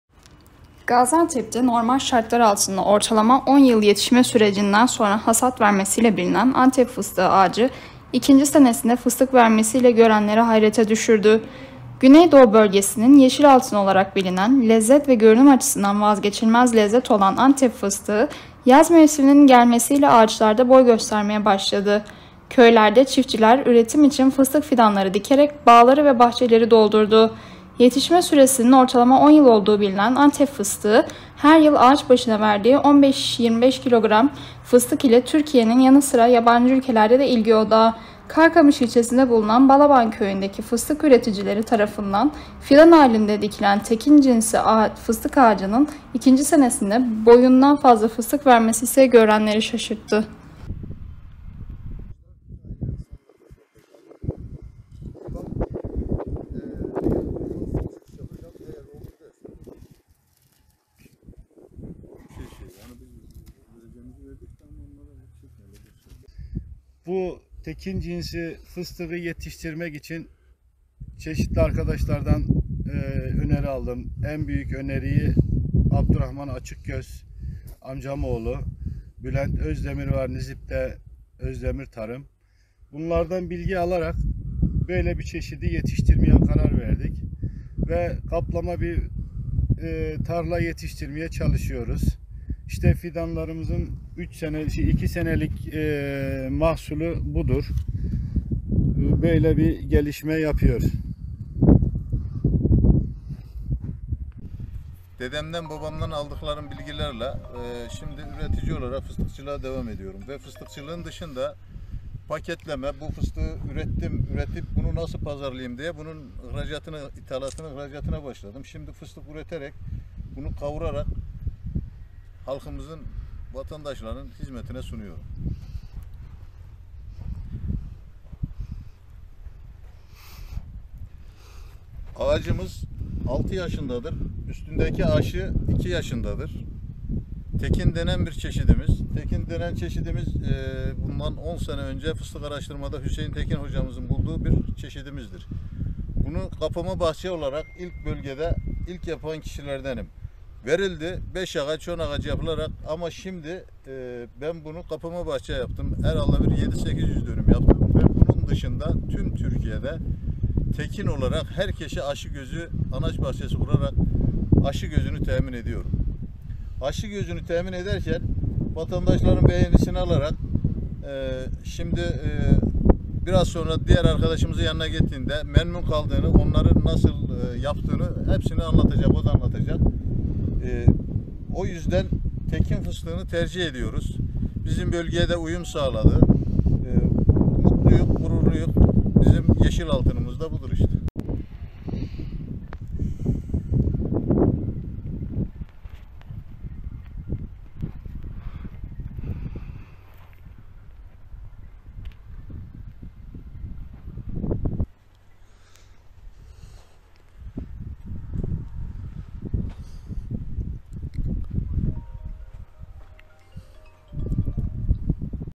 Gaziantep’te fıstık üreticileri gözlerine inanamadı
Ortalama 10 yılda meyve veren fıstık ağacı türlerinin aksine yeni tür 2 yılda meyve verdi.